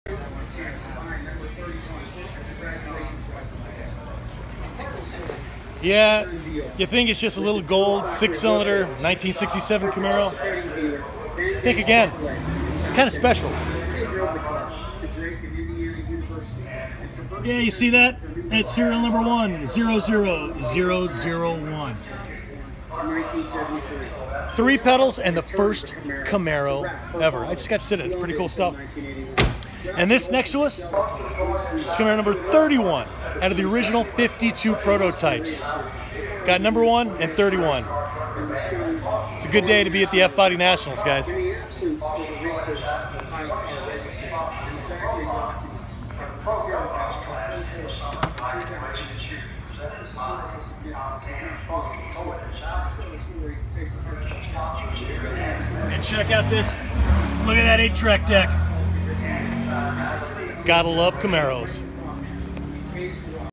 The first Camaro - Serial Number 100001 & Camaro #31 - at the F-Body Nationals.
The first Camaro - Serial Number 100001 - at the F-Body Nationals.
Built in May of 1966, it's the first ever Camaro and it's got a 6-cylinder under the hood.
Oh yeah - we've also got Camaro #31 here too